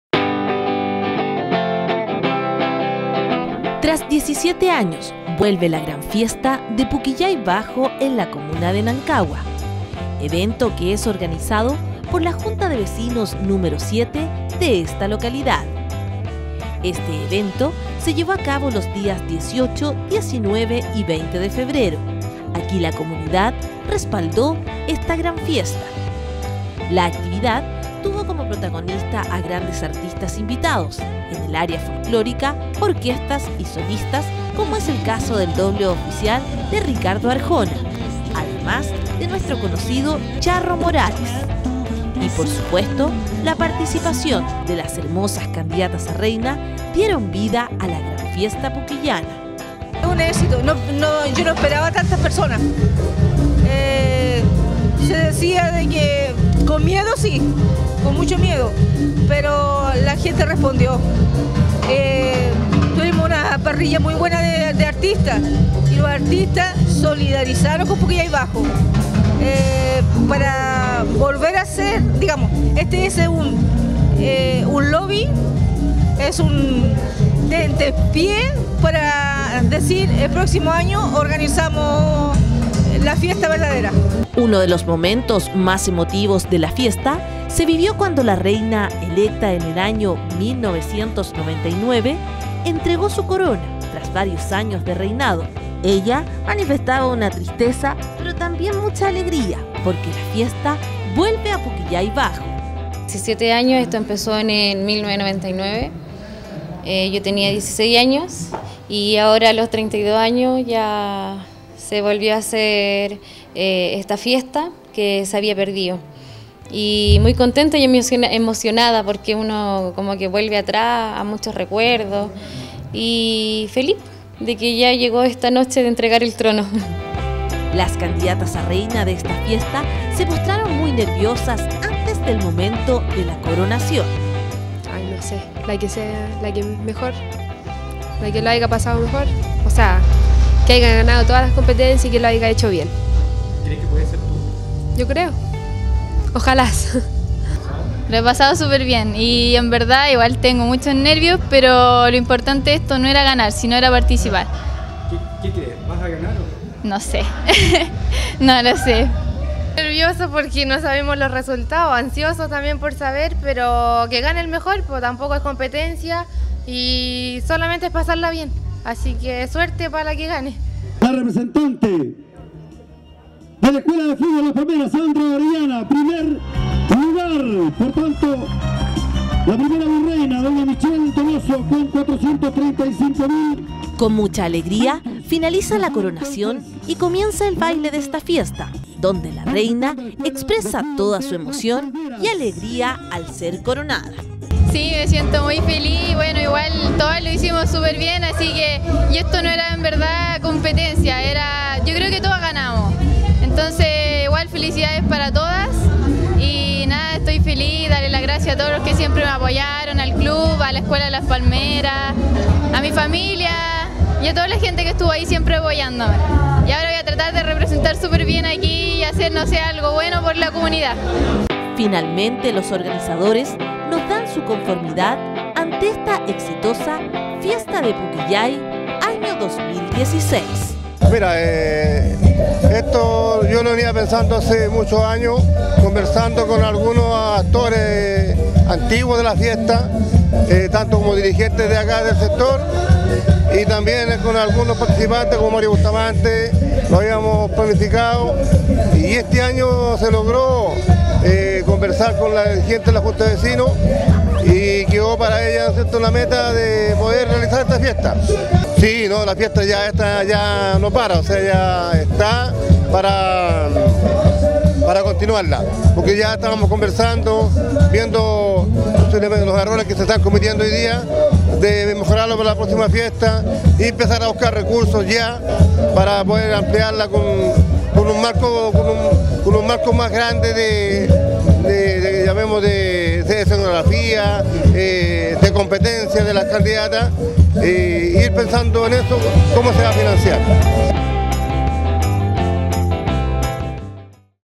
FIESTA PUQUILLAYINA 2016
En Nancagua...Felicitaciones a los vecinos de Puquillay quienes retoman una gran celebración después de 17 años.